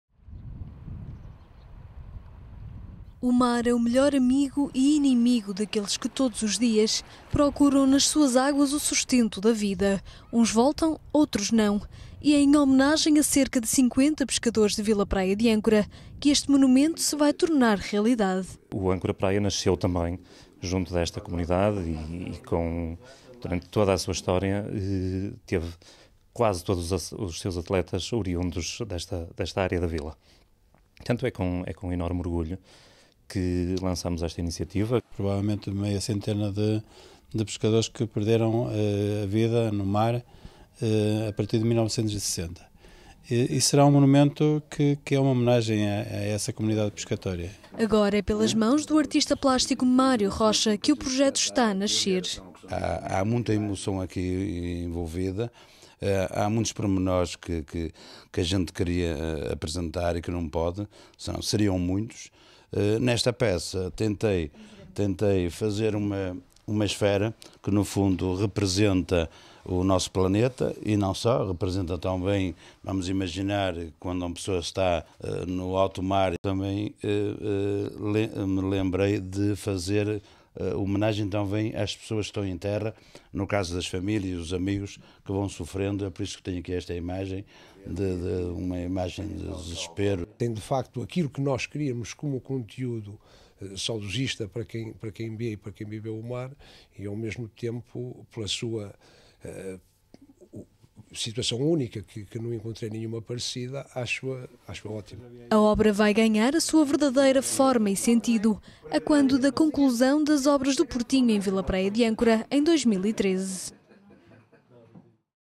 Homenagem a pescadores de Vila Praia de Âncora Caminha Porto Canal
Uma homenagem aos homens do mar  de Vila Praia de Âncora e que nele perderam a vida. Uma obra criada por Mário Rocha.

Jornalista: Andreia Alexandra Fernandes
Imagem: Miguel Azevedo